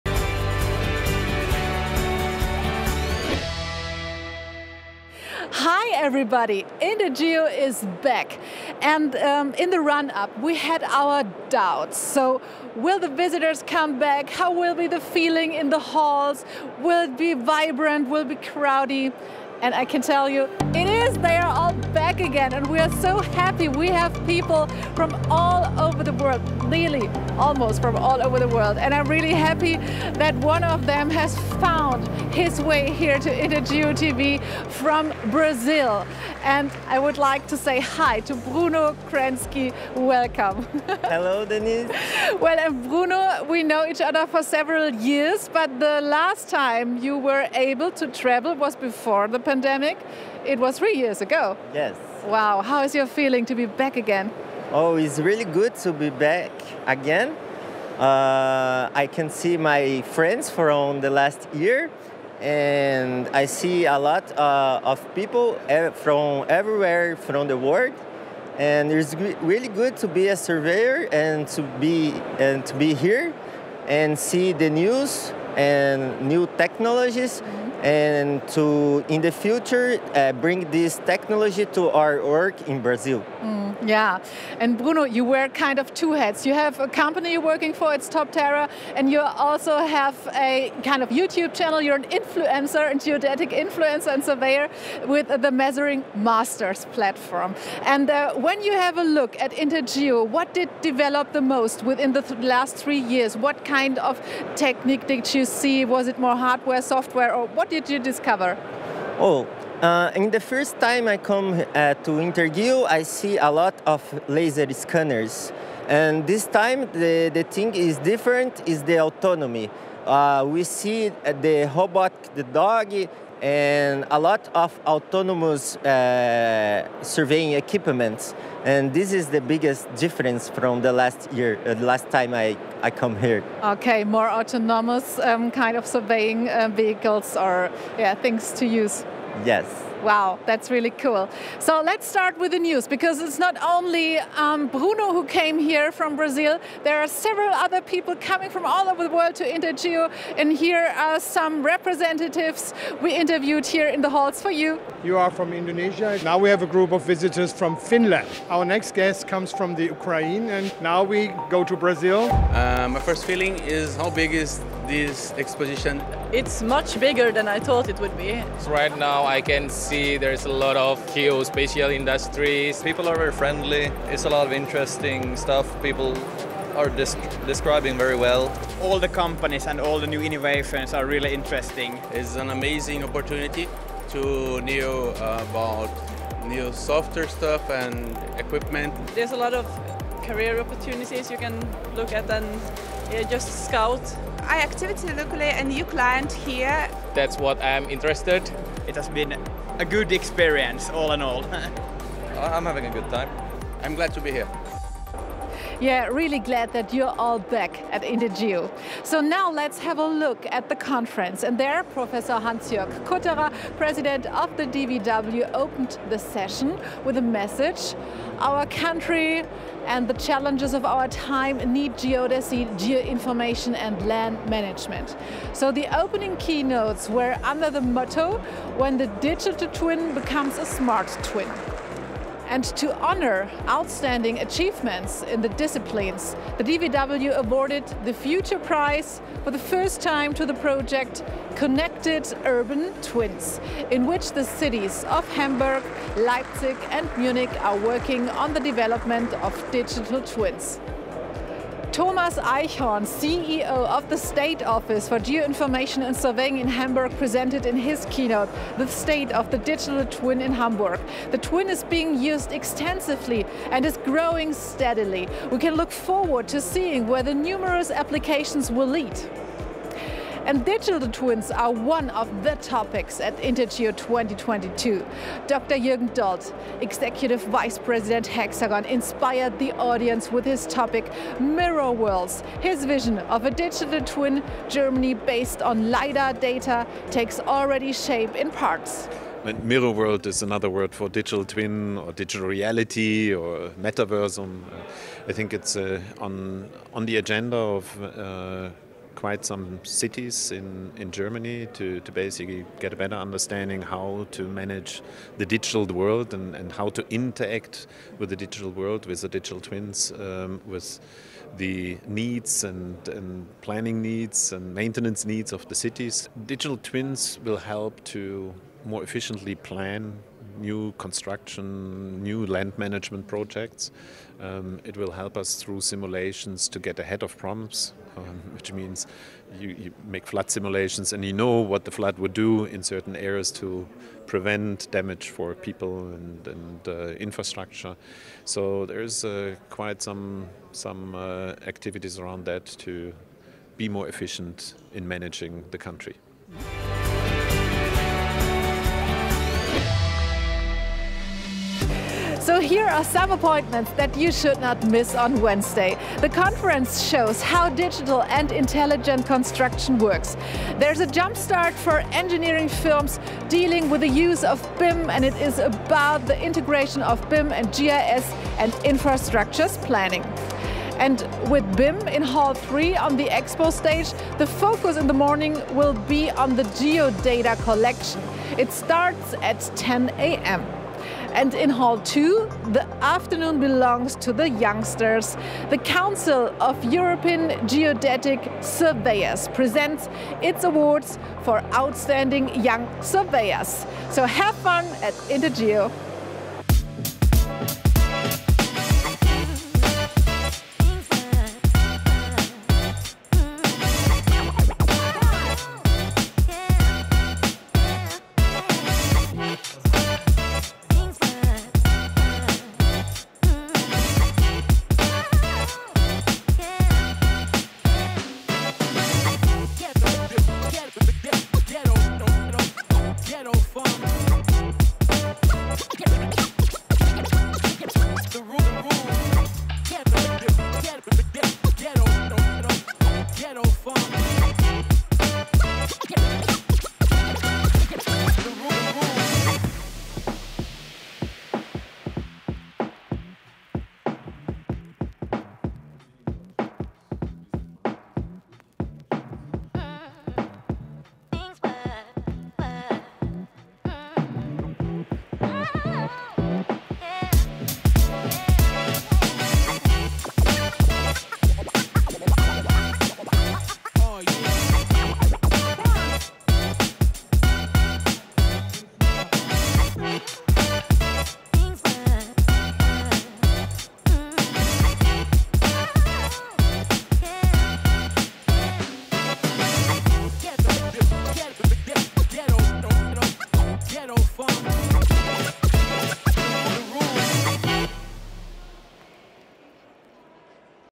INTERGEO TV NEWS | INTERGEO 2022 Day 1!
INTERGEO in Essen, the news of the day. Including: Opening of the event with DVW President Prof. Dr. Hansjörg Kutterer, keynotes by Dr. Jürgen Dold and Thomas Eichhorn. International voices on the fair! District President of Cologne, Dr. Thomas Wilk, visits INTERGEO and lots of impressions.

INTERGEO in Essen, die News des Tages. Mit dabei: Opening der Veranstaltung mit DVW-Präsident Prof. Dr. Hansjörg Kutterer, Keynotes von Dr. Jürgen Dold und Thomas Eichhorn. Internationalen Stimmen zur Messe! Regierungspräsident von Köln, Dr. Thomas Wilk, besucht die INTERGEO und jede Menge Impressionen.